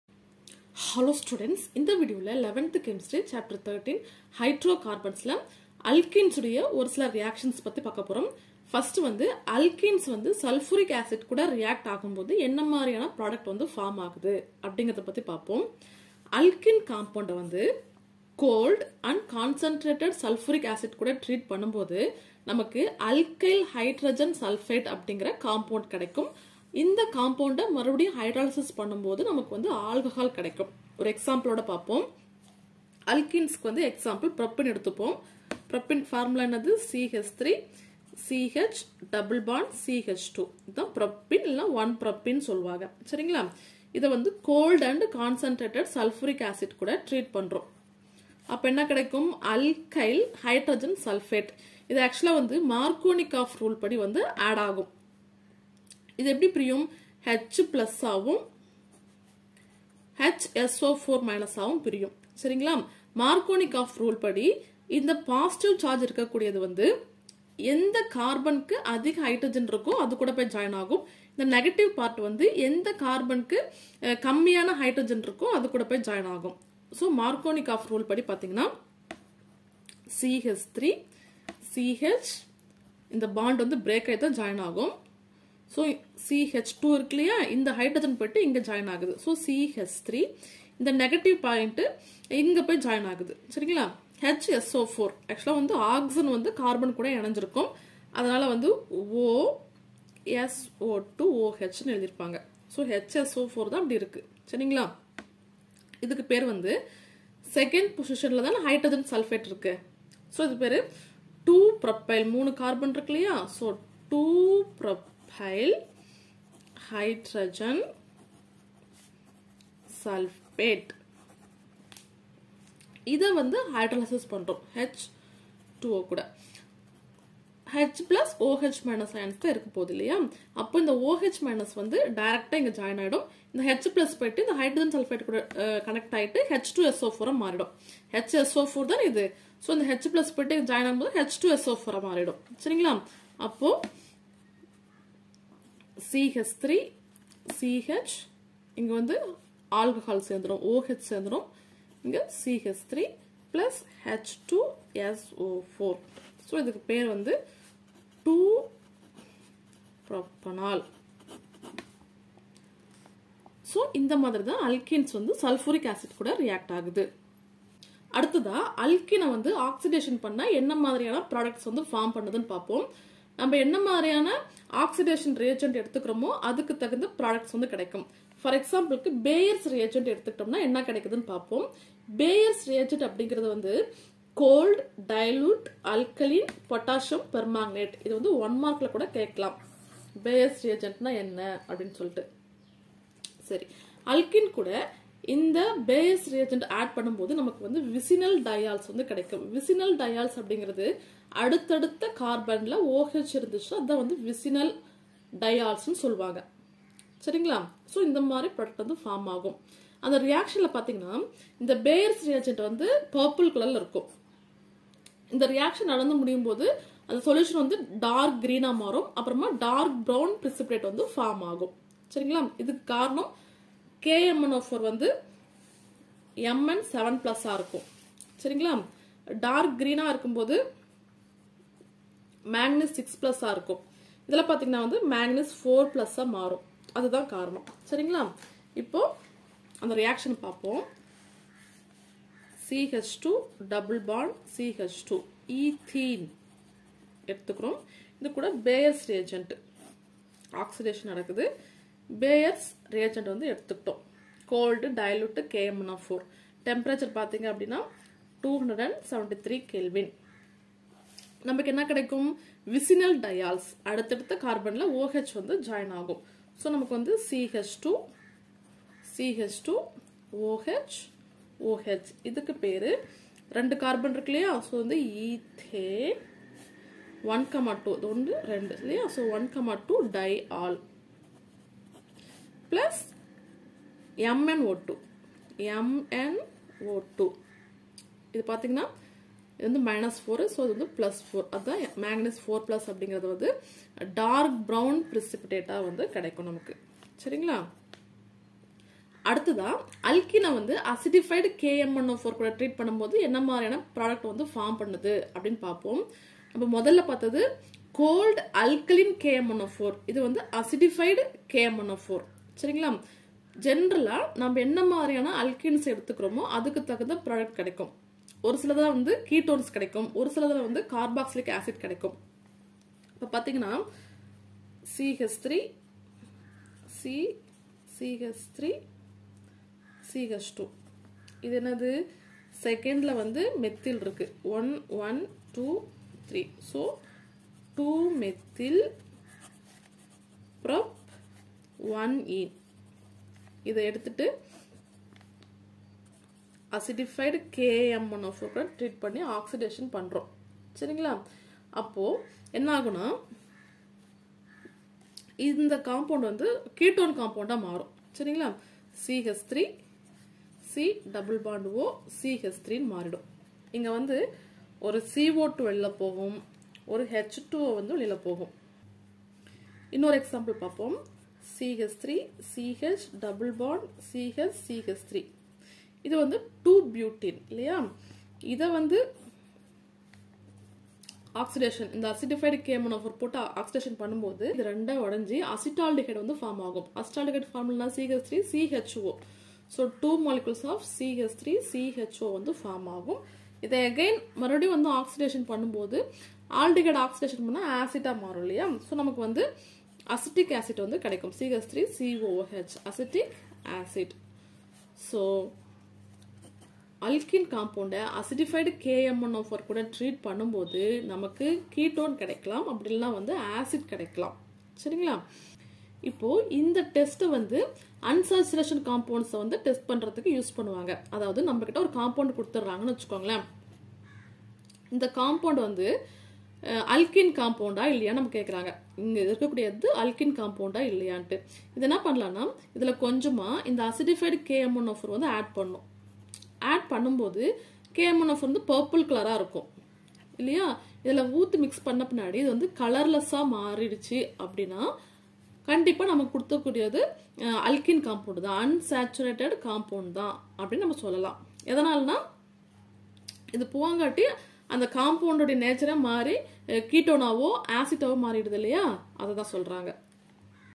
Oxidation & addition of sulphuric acid to alkenes | peroxide effect / Hydrocarbons | Unit - 13
Oxidation & addition of sulphuric acid to alkenes | peroxide effect / Hydrocarbons | Unit - 13 | Class 11 chemistry|inTamil